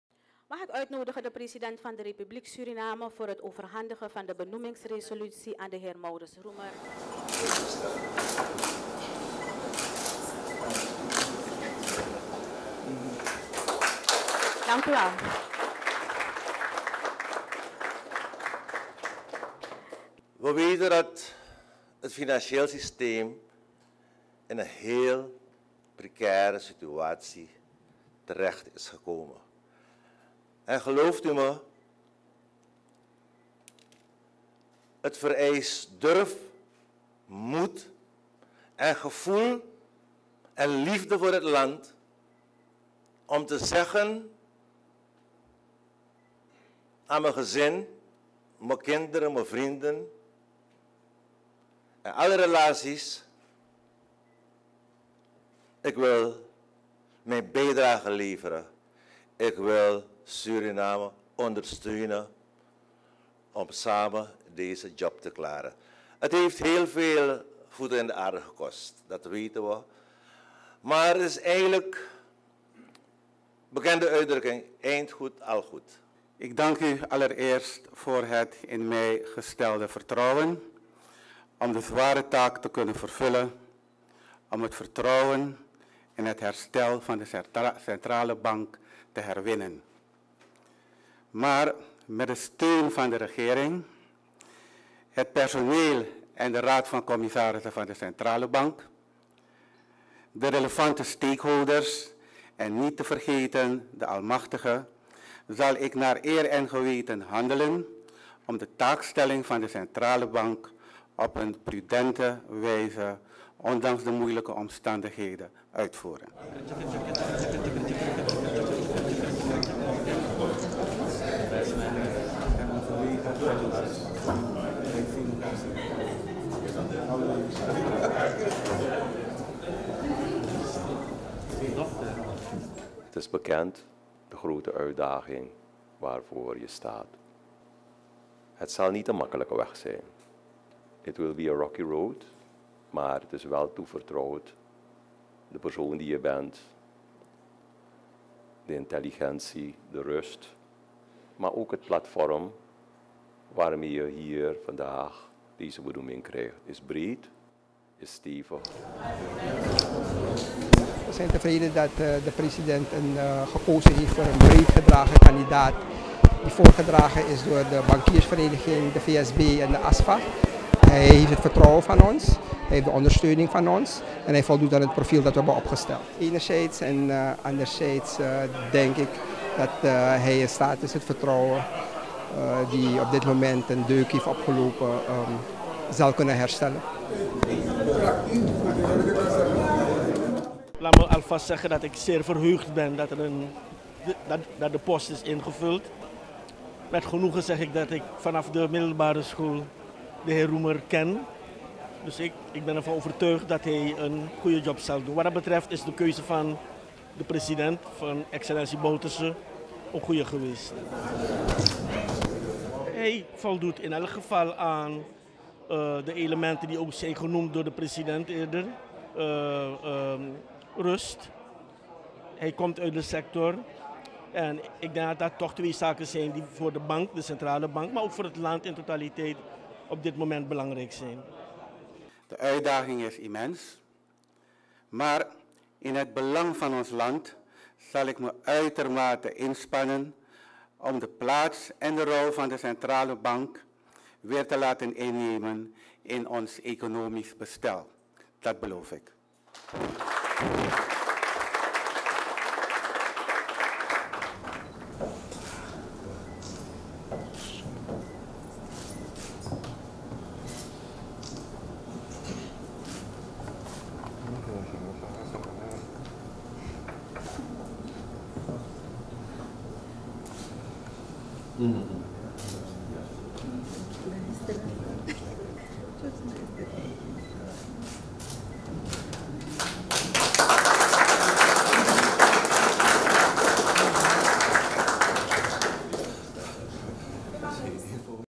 Maurice Roemer, governor Centrale Bank van Suriname
StarNieuws Suriname

Op Maurice Roemer, de nieuwe governor van de Centrale Bank, rust een zware taak om “het vertrouwen en herstel van de Centrale bank te herwinnen. Hij belooft zijn zich "uitermate" in te spannen om dat doel te bereiken.
Category
People & Blogs
License